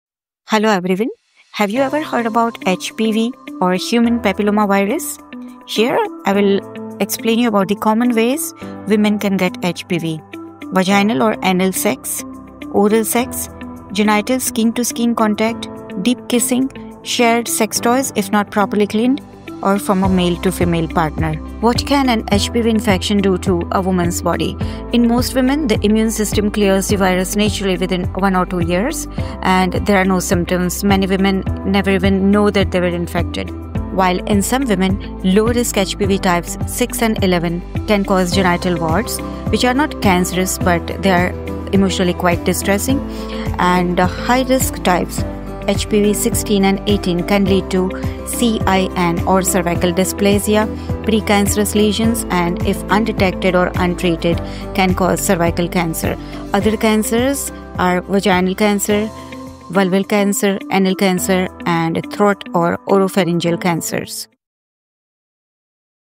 HPV in Women: Hidden Dangers & Early Signs You Shouldn’t Ignore!
HPV, or Human Papillomavirus, is one of the most common sexually transmitted infections affecting women worldwide. In this video, you’ll learn how women get HPV, the difference between low-risk HPV and high-risk HPV, and the possible health complications it can cause — including cervical dysplasia, precancerous lesions, and even cervical cancer.

HPV is mainly spread through:

Vaginal, anal, or oral sex

Genital skin-to-skin contact

Deep kissing

Shared, uncleaned sex toys

From male to female partner

While most HPV infections clear naturally within 1–2 years with no symptoms, some types (HPV 6 & 11) cause genital warts, and high-risk types (HPV 16 & 18) can lead to serious health issues like cervical, vaginal, vulvar, anal, and throat cancers.

Stay informed, take preventive measures, and get regular health checkups to detect any problems early.